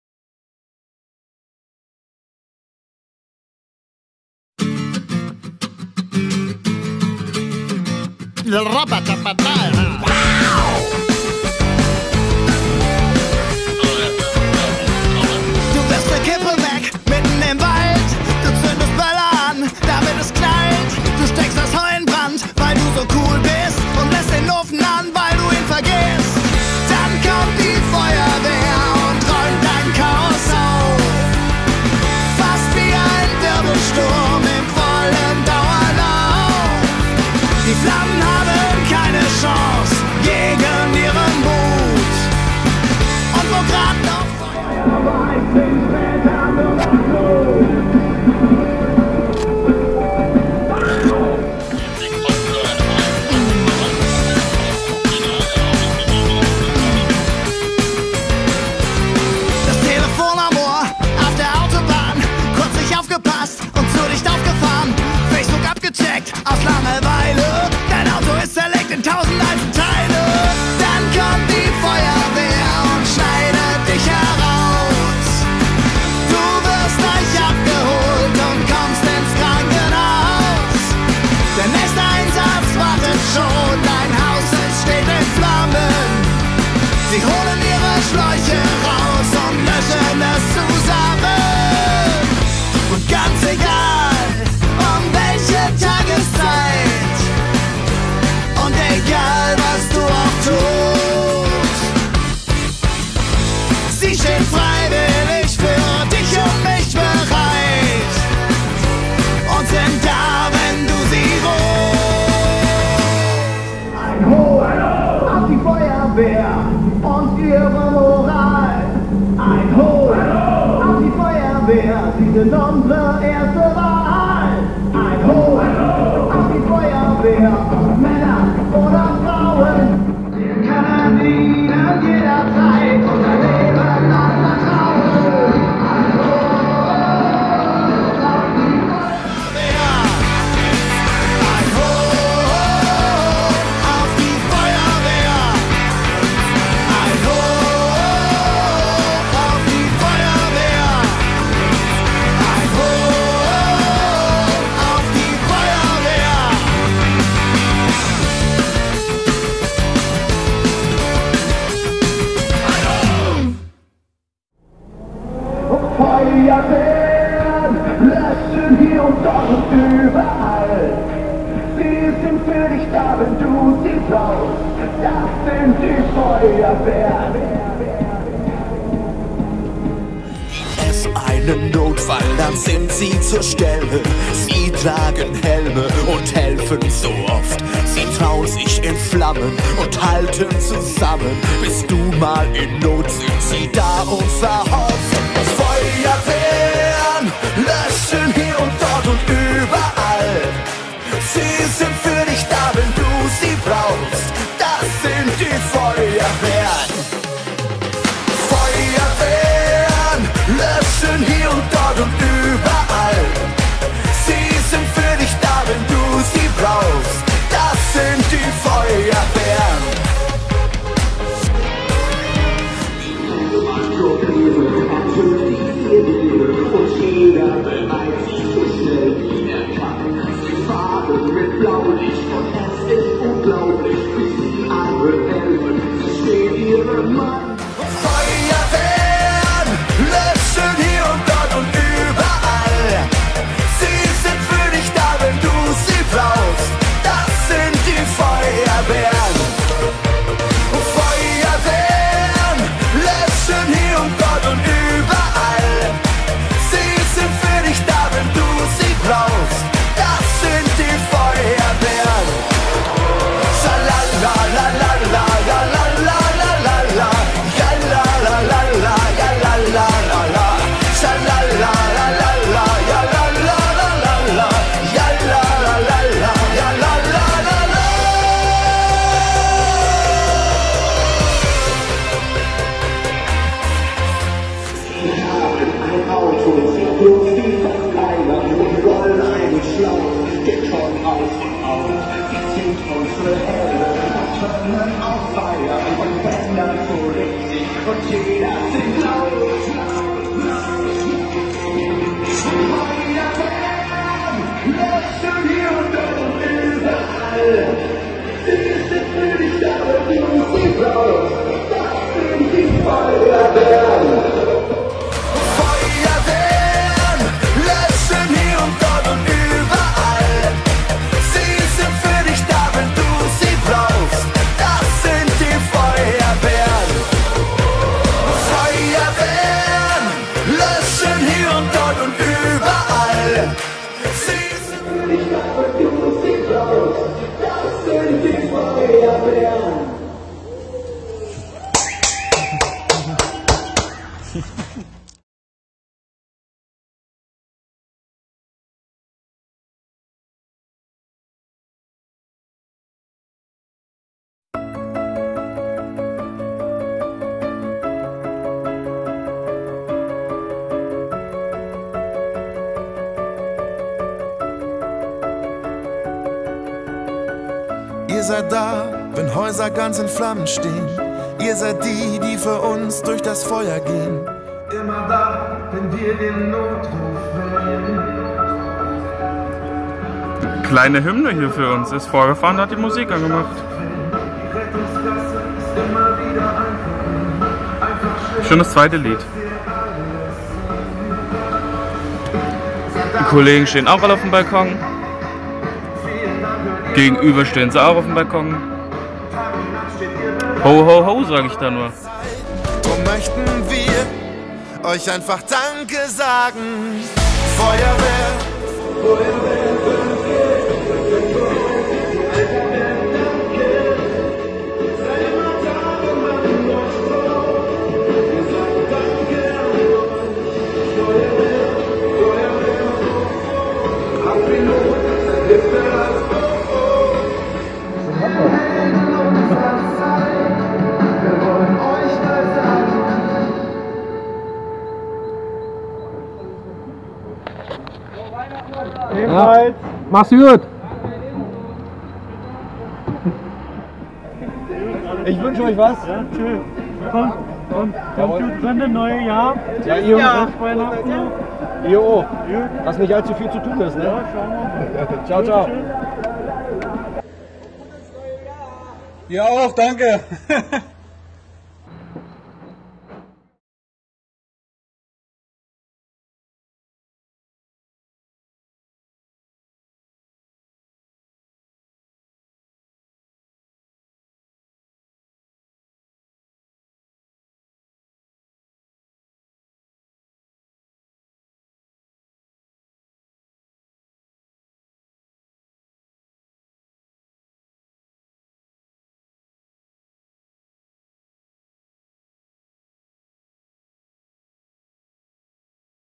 Danke Feuerwehr (Weihnachten 2019)
An den Weihnachtsfeiertagen habe ich mir gern wieder die Zeit genommen, um einen musikalischen Gruß an die Feuerwehren in Berlin zu verteilen.
Dieses Jahr gibt es darüber einen Film, zu dem einige Feuerwehrleute Filmmaterial beigetragen haben. so wirkt das besonders toll, Dankeschön!
Mit den Zuordnungen der Wachen bin ich letztendlich doch durcheinandergekommen. machen wir doch ein Suchspiel daraus, Freunde ;)
Schreibt in die Kommentare, welche Feuerwachen ihr erkannt habt!
Ich freue mich auf regen Austausch.